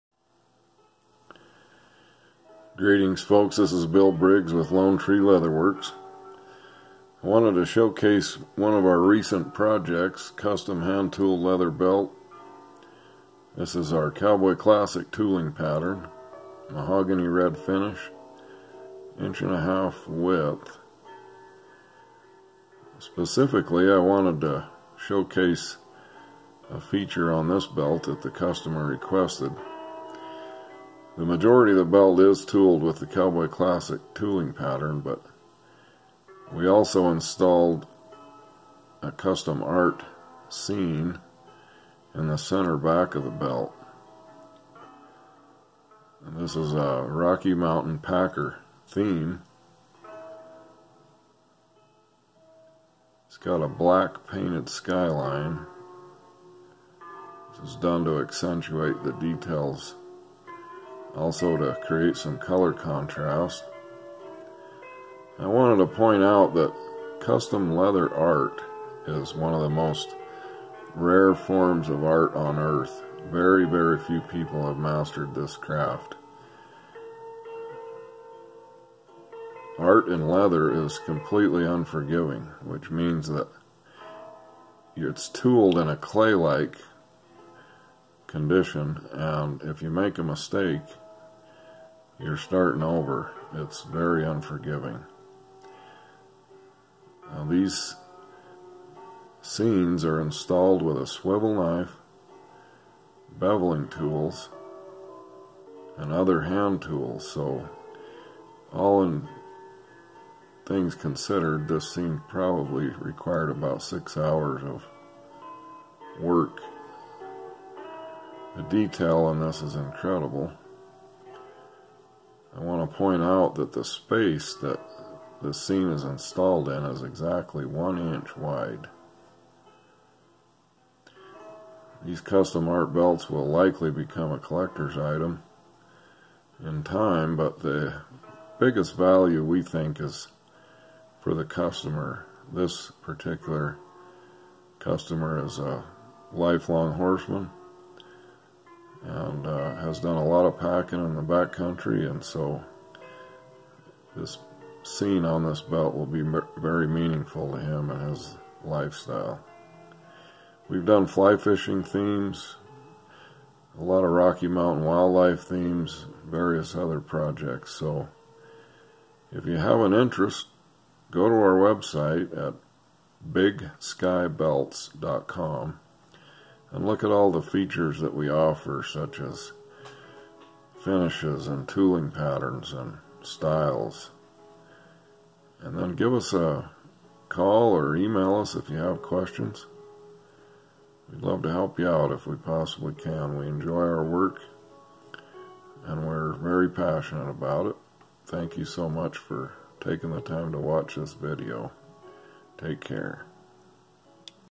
Hand tooled custom art belt
Genuine hand tooled custom art belt hand crafted by Montana cowboys.  Rocky Mountain Packer theme.  Mahogany red finish.  Cowboy Classic tooling pattern.